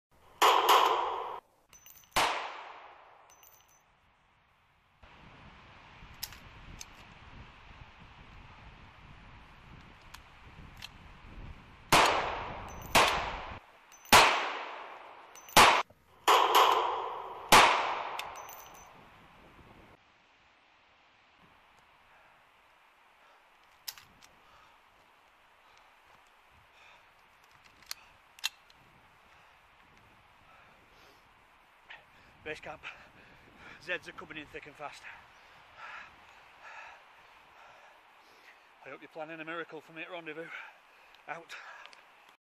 Day 320 short film. Gunfire clip.
Alan is surrounded and running out of ammo.